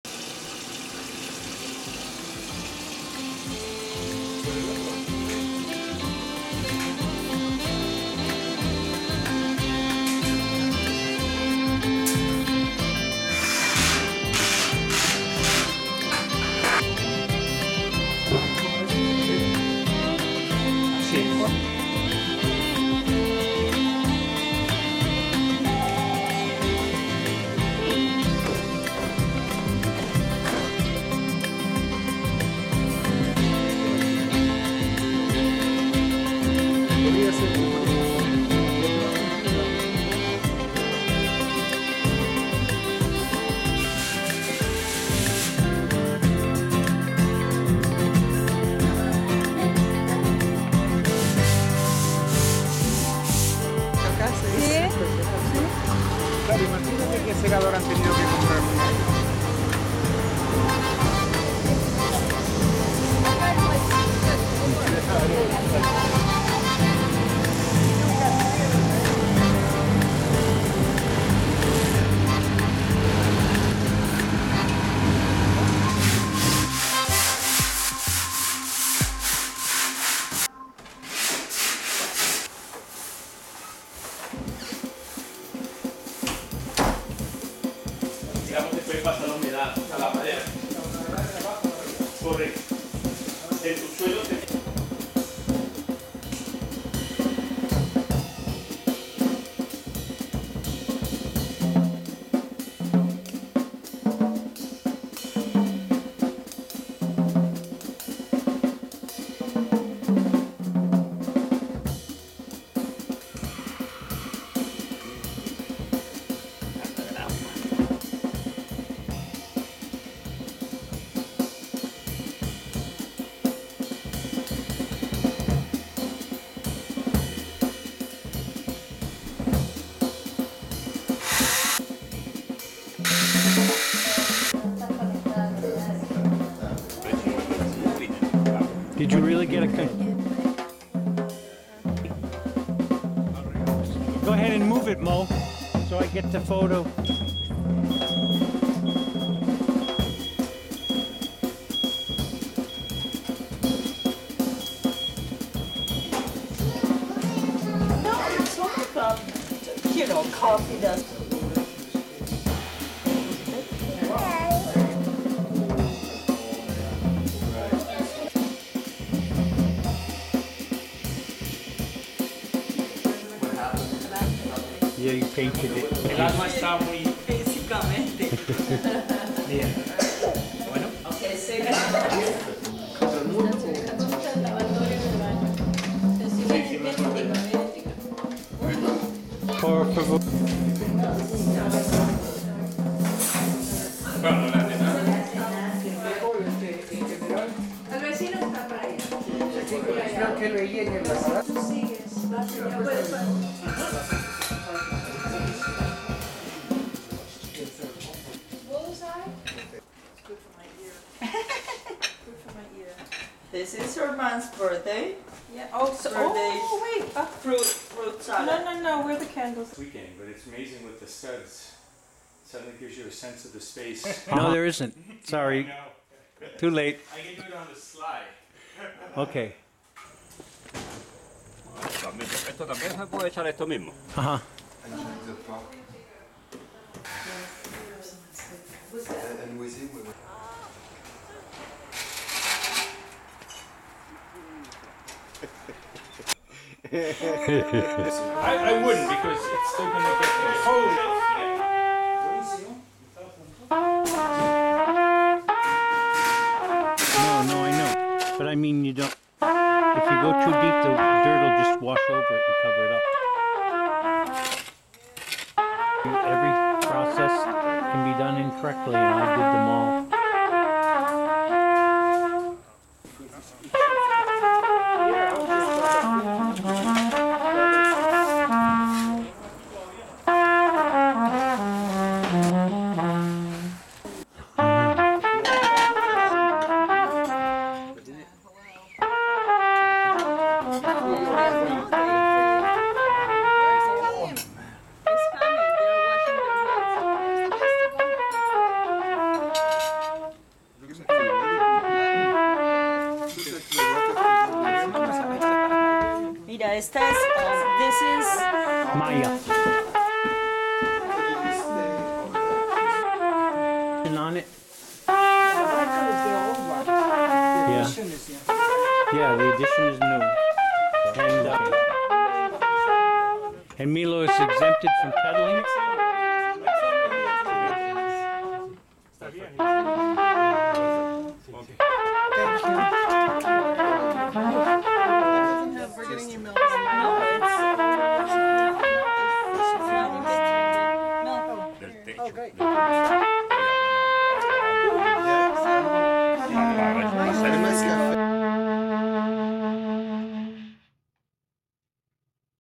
A Weekend at Hudson Valley Park of Study and Reflection.mpg
Last weekend, the end of August, many diffent people came and went in order to help fix our Park.
This is a short video of our  joys and efforts.
It is dedicated to Quim, as a way of thanking him for all his help.